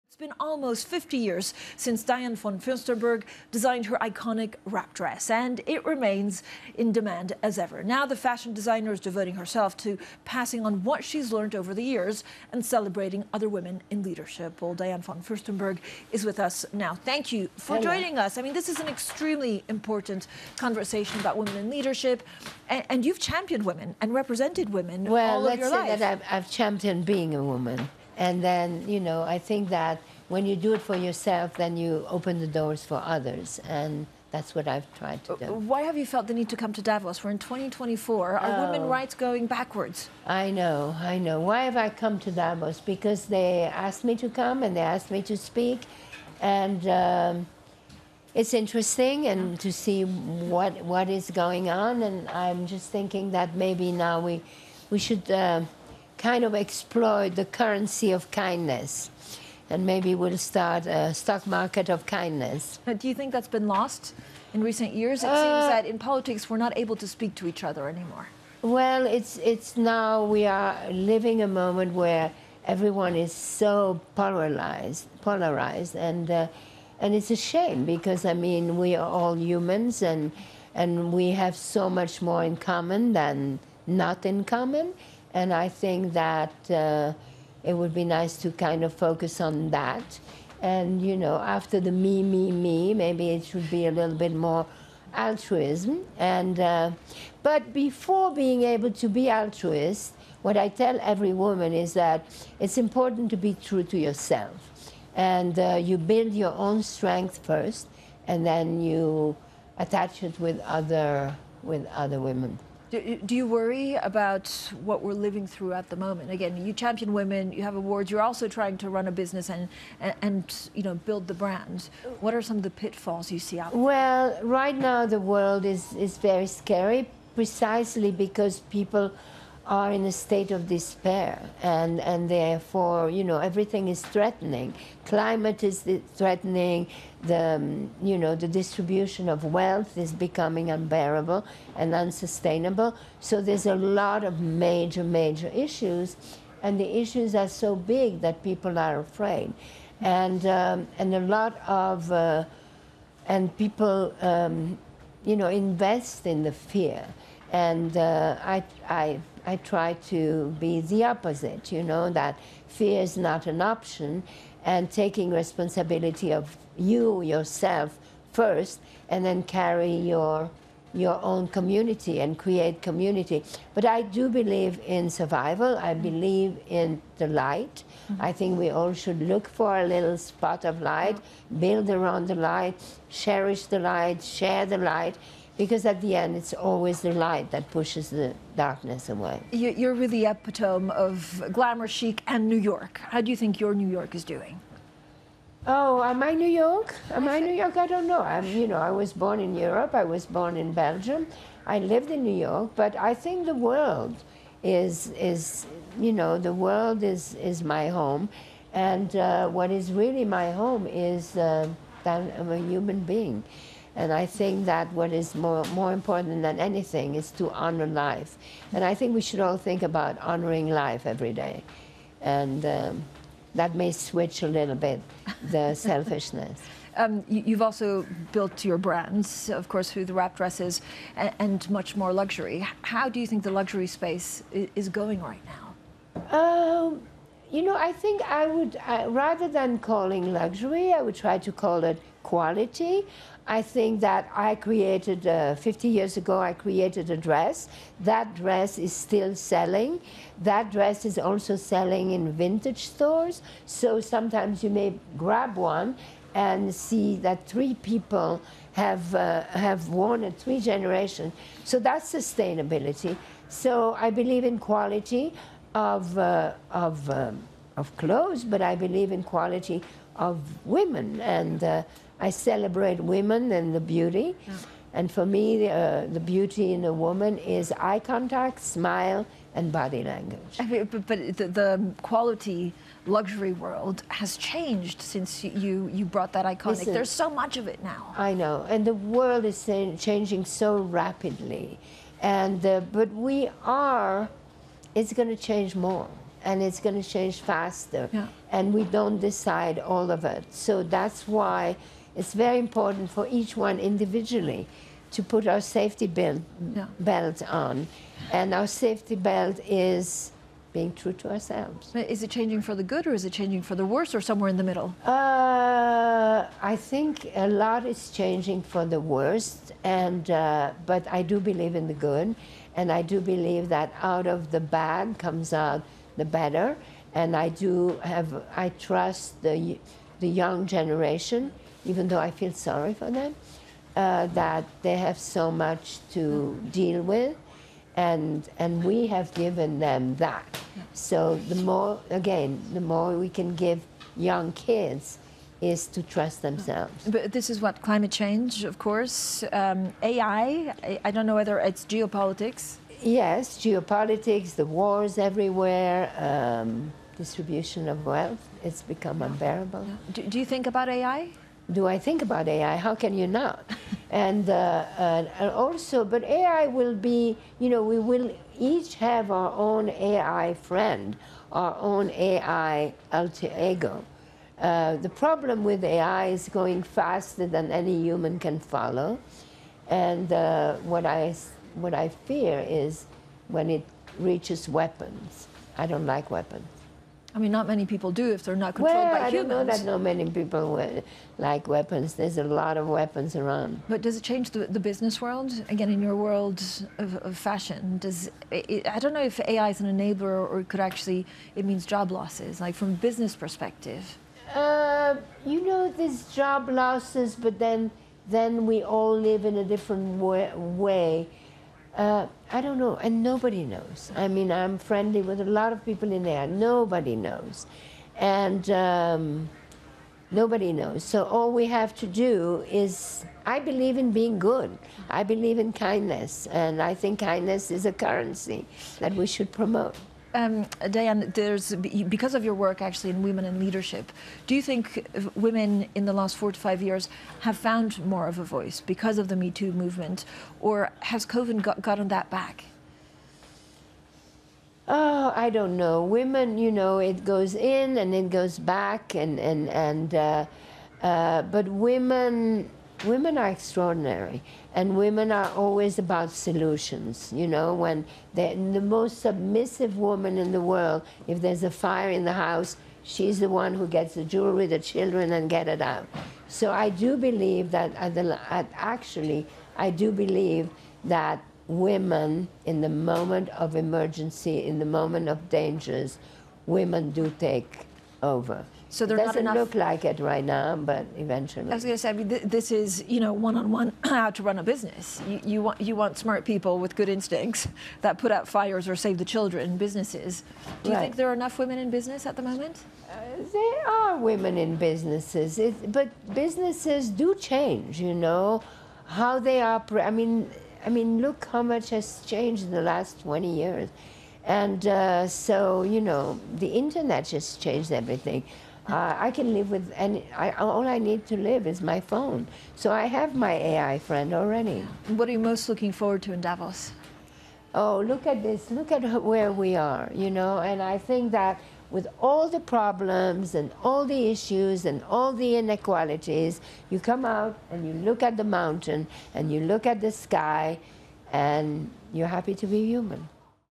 Diane von Furstenberg on Fashion, Women, AI: Davos 2024
Diane von Furstenberg discusses running her business ami geopolitical challenges and her thoughts on the future of fashion and AI. She speaks on Bloomberg Television at the World Economic Forum's annual meeting in Davos, Switzerland.